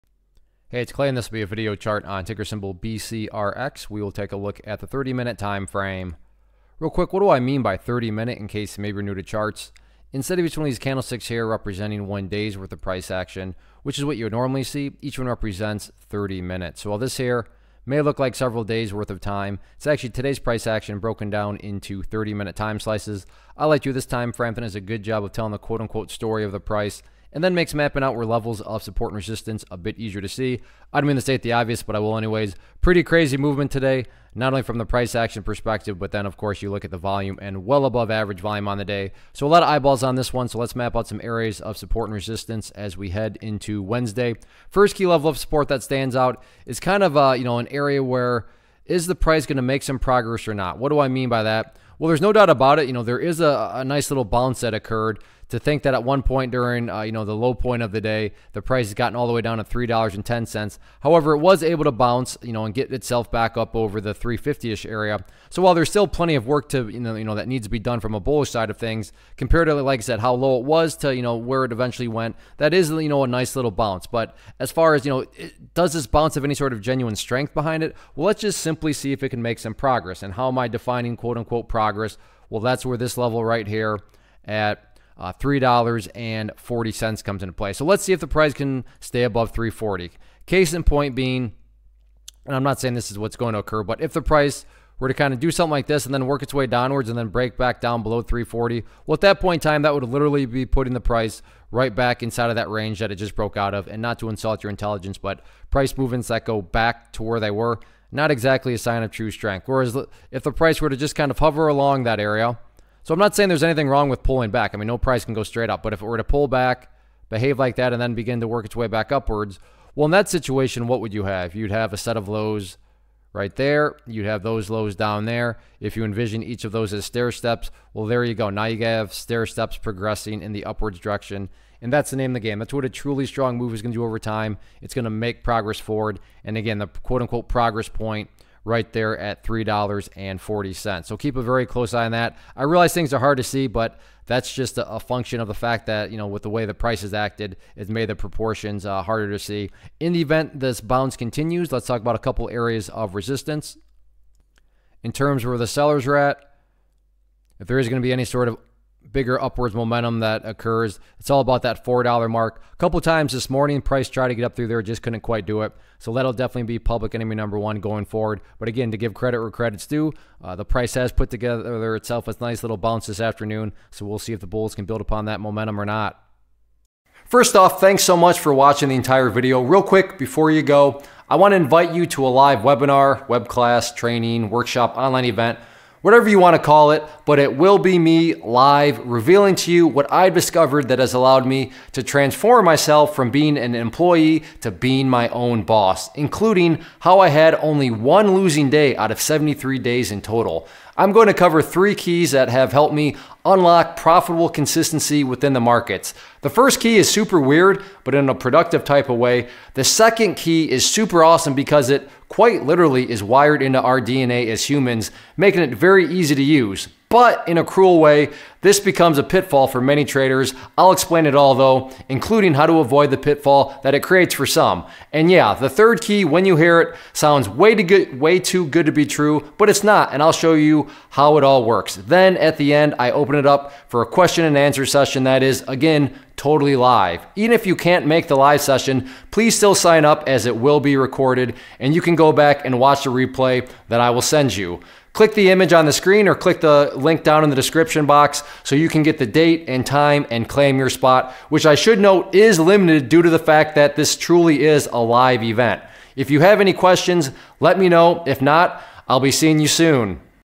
BioCryst Pharmaceuticals, Inc. - BCRX Stock Chart Technical Analysis for 05-21-2019
Learn how to read stock charts and identify technical patterns as ClayTrader does a quick stock chart review on BioCryst Pharmaceuticals, Inc. (BCRX).